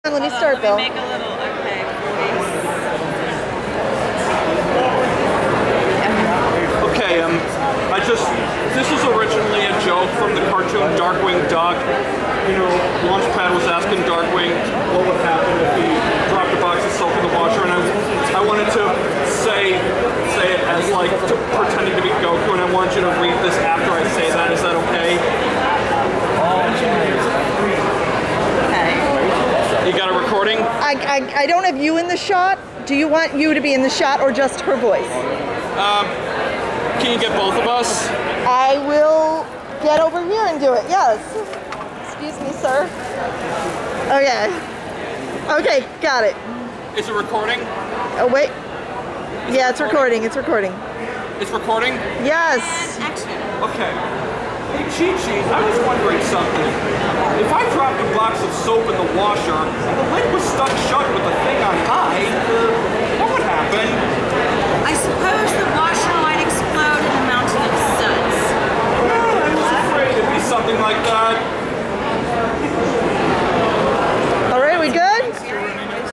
Cynthia Cranz (Anime Midwest 2021)
With my mom filming us, I asked the voice of Chichi from DBZ to do a bit with me where we quote the washing machine dialogue that Launchpad and Drake said from the Darkwing Duck episode, Comic Book Capers! XD XD XD